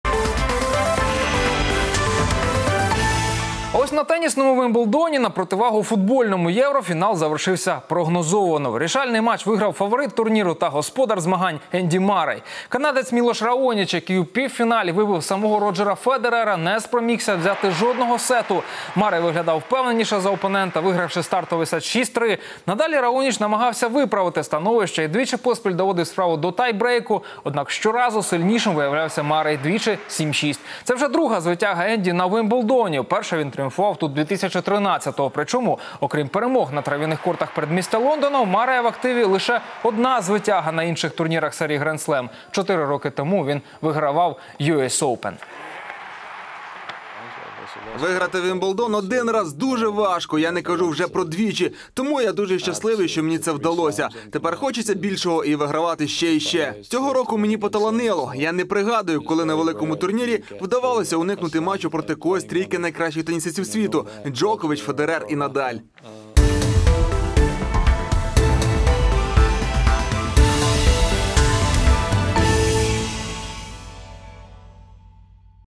Энди Маррей второй раз стал чемпионом Уимблдона
На теннисном Уимблдоне в противовес футбольному Евро финал завершился прогнозируемо.
На тенісному Вімблдоні на противагу футбольному Євро фінал завершився прогнозовано.